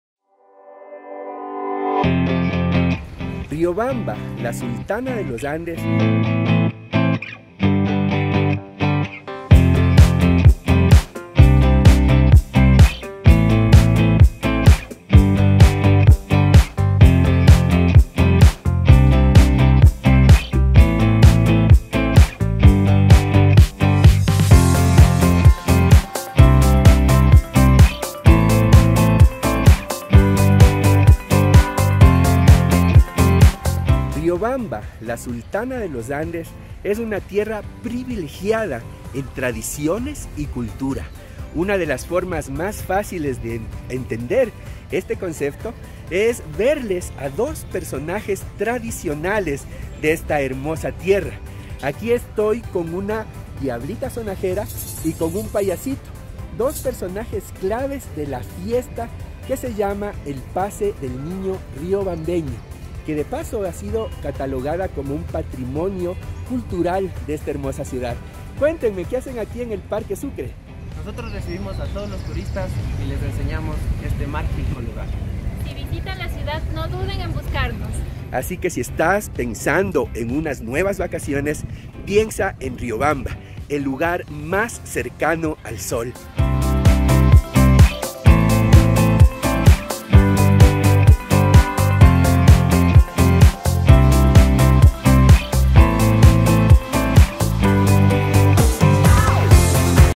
Pases de niño con PAYANI, un forma de hacer turismo y conocer las tradiciones de Riobamba.
Goraymi te muestra lo fascinante que resulta hacer turismo en Riobamba al conocer una de sus tradiciones más importantes que son los pases de niño y con peculiares personajes que  harán de tu experiencia algo maravilloso.
Sé parte de los héroes del turismo.